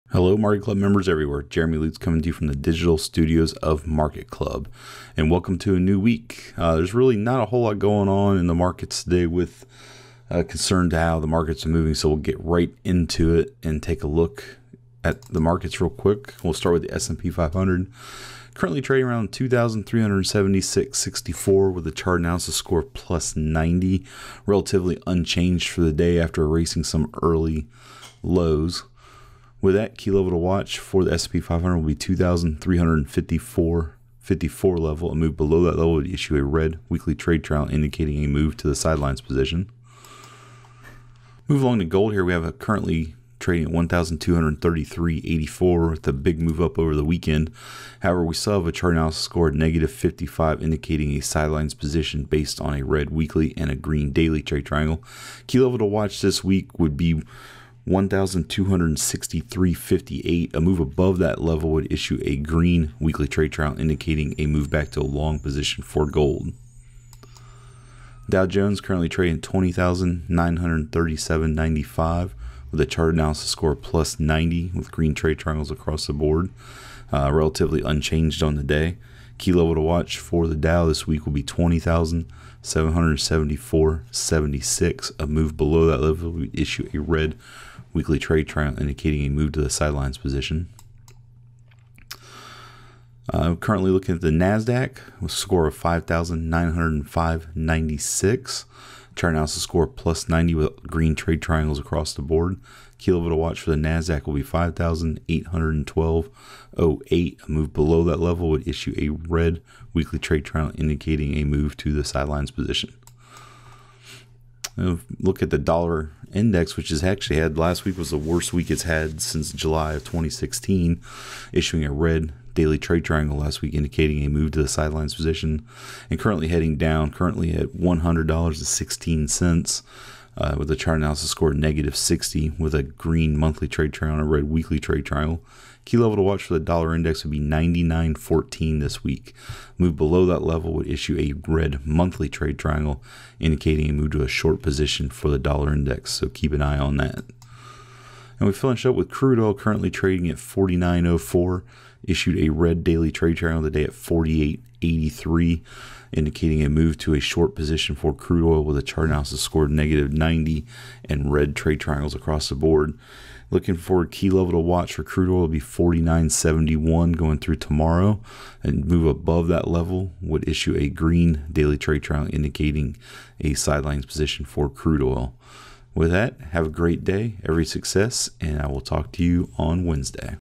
U.S. Dollar Slips As Stocks Fluctuate
Hello MarketClub members everywhere. The market is relatively unchanged today after erasing earlier losses as real estate and technology turned higher and traders turned their eyes to comments from several Federal Reserve officials.

The U.S. central bank raised interest rates for the second time in three months last week, but the "dot plot" that shows each member's expectations for where rates will be in coming years changed little from the last meeting.

Key levels to watch this week:
S&P 500 (CME:SP500): 2,354.54
Dow (INDEX:DJI): 20,774.76
NASDAQ (NASDAQ:COMP): 5,812.08
Gold (FOREX:XAUUSDO): 1,263.58
Crude Oil (NYMEX:CL.J17.E): 49.71
U.S. Dollar (NYBOT:DX.M17.E): 99.14